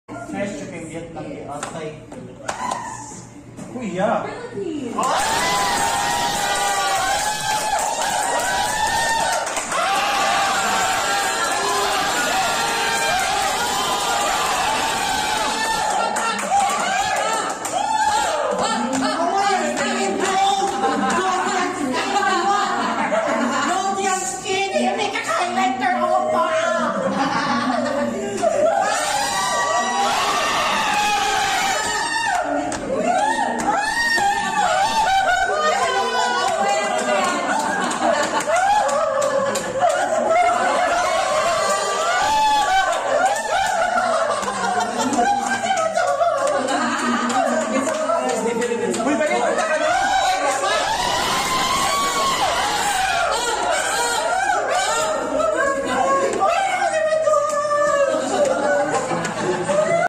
TOP 21 MISS UNIVERSE 2020 REACTION! (PHILIPPINES)
Our reaction during the announcement of Top 21 Miss Philippines Rabiya Mateo!!!
We are so proud of you Rabiya! Thank you for representing our country 🇵🇭🇵🇭🇵🇭